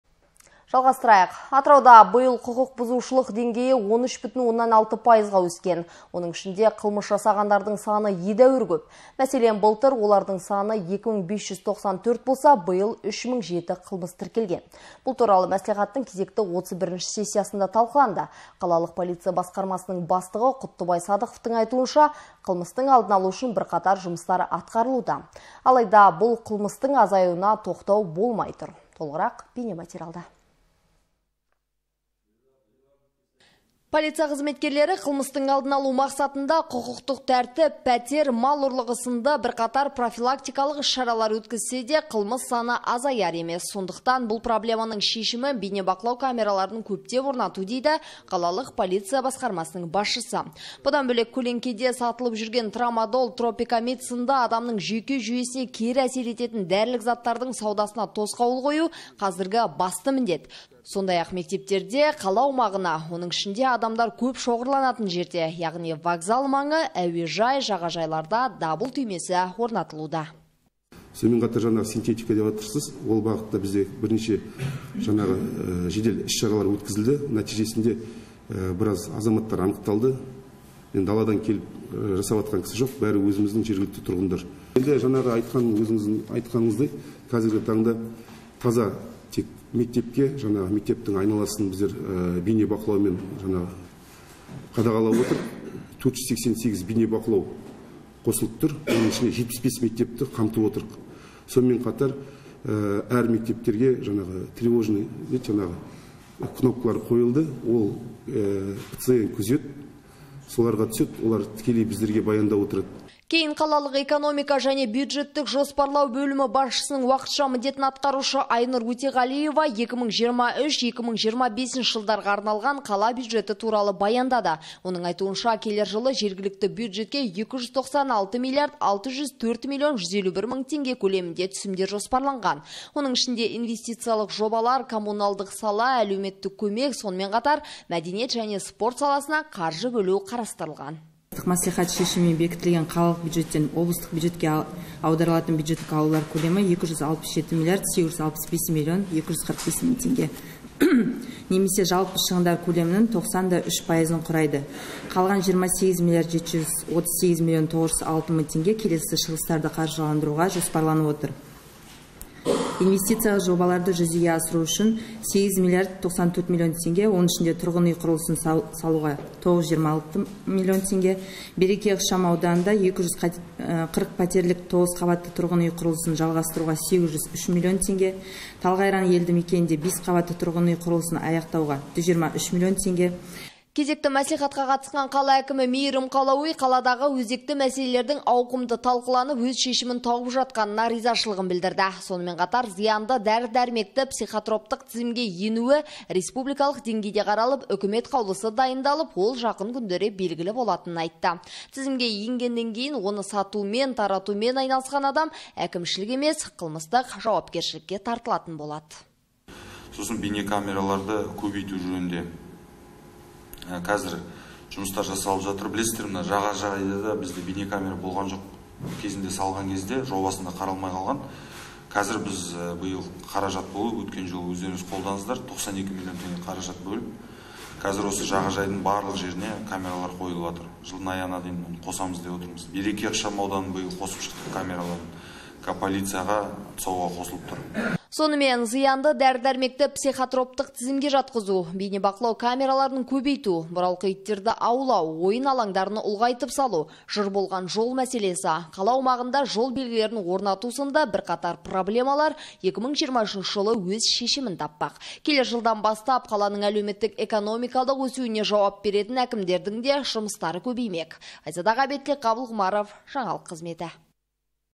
ҚЫЛМЫС АЗАЙМАЙ ТҰР
Атырауда биыл құқық бұзушылық деңгейі 13,6 пайызға өскен. Оның ішінде қылмыс жасағандардың саны едәуір көп. Мәселен, былтыр олардың саны 2594 болса, биыл 3007 қылмыс тіркелген.  Бұл туралы мәслихаттың кезекті 31-ші сессиясында талқыланды. Қалалық полиция басқармасының бастығы Құттыбай Садықовтың айтуынша, қылмыстың алдын алу үшін бірқатар жұмыстар атқарылуда. Алайда, бұл қылмыстың азаюына тоқтау болмай тұр. Толығырақ бейнематериалда.